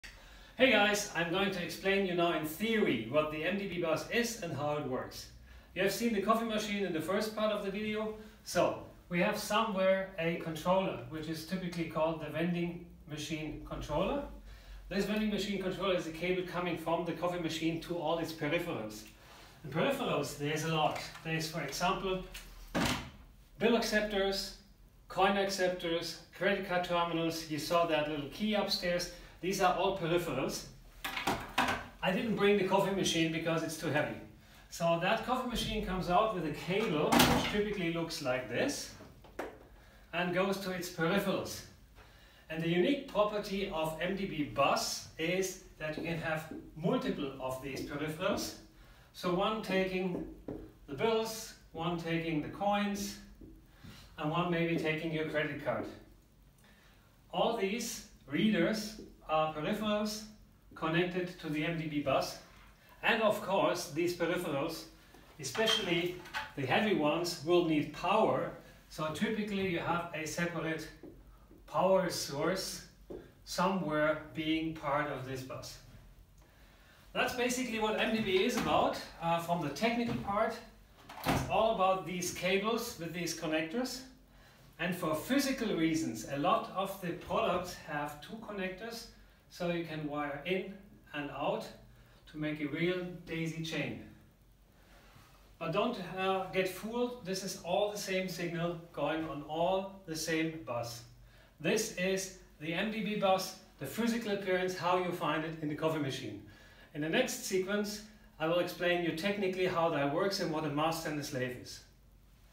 MDB explained
MDB-USB Interface basic explanation of the functionality in a vending machine.